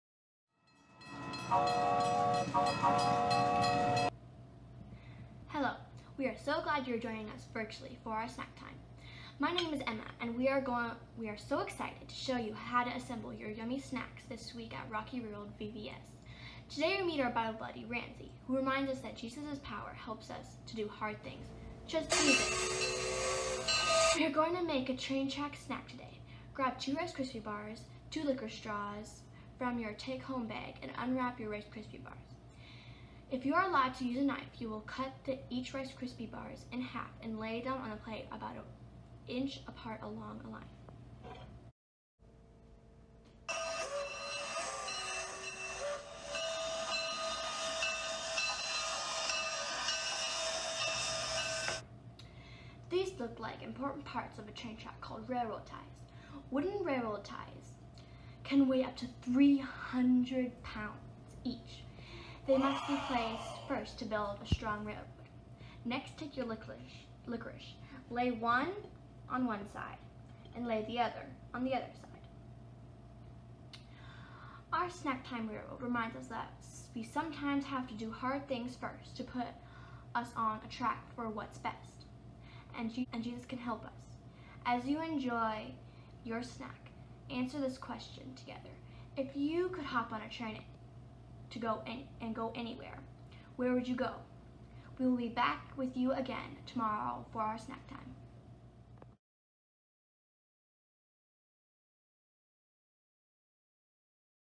Chew Chew Snacks Day 1 - Trinity VBS
Join conductor Ms. Emma and learn how to make a delicious Chew Chew Track snack.

Visit TrinityLongLake.org/VBS for more Rocky Railway videos and activities as we ride the rails for a wild adventure this week!